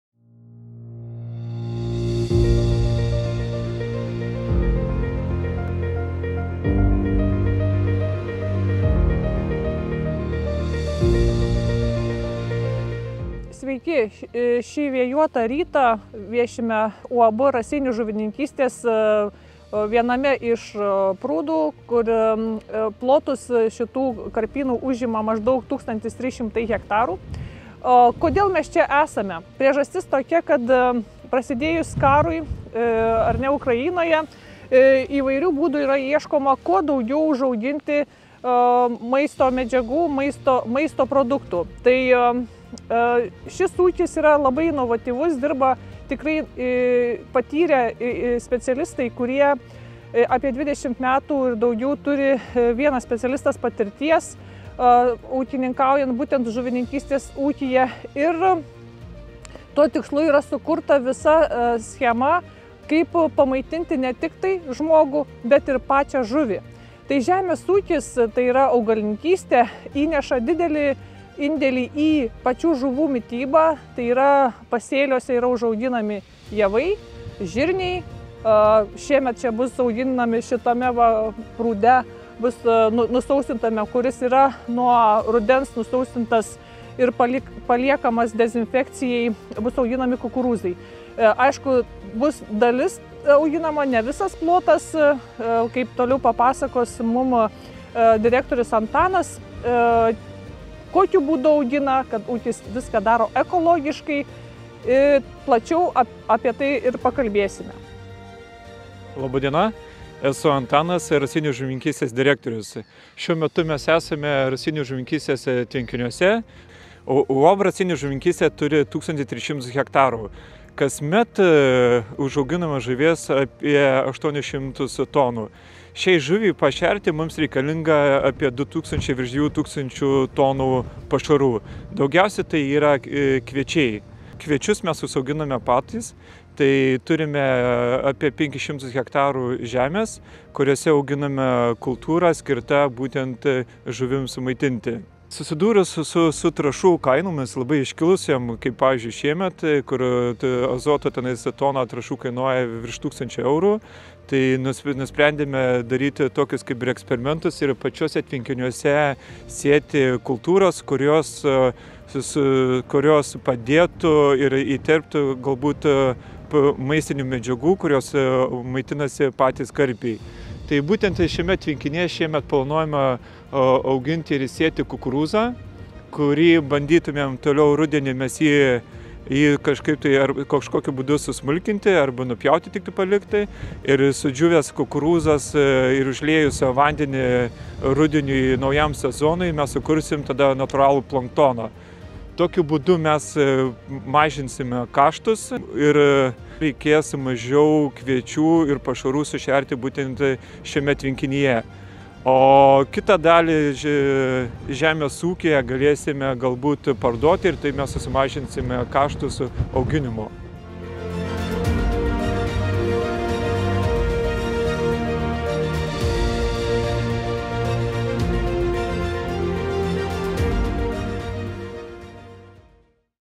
Žuvininkystės ūkis
Apie tai kaip žemės ūkis susijęs su žuvininkystę pasakoja AB Kauno grūdai agrotechnologijų plėtros ir produktų vadovė dr. Gabrielė Pšibišauskienė, bei UAB "Raseinių žuvininkystė" direktorius Antanas Grigalavičius.